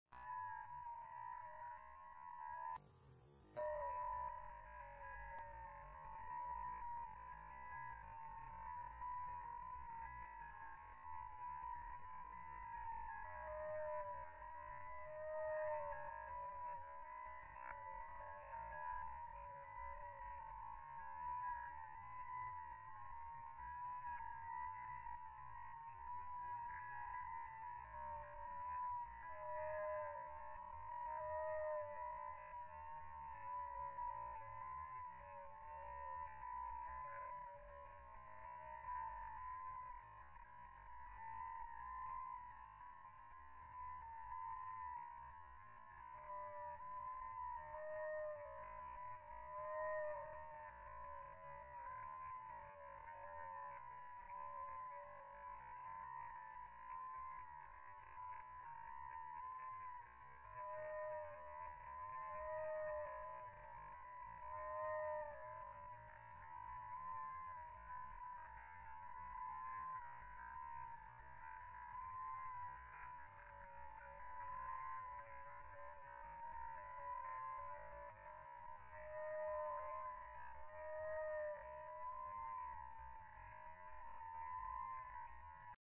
Bumblebee missing its flower in slow motion.
A slow motion video of a Bumblebee flying and appears to miss the flower it was heading to.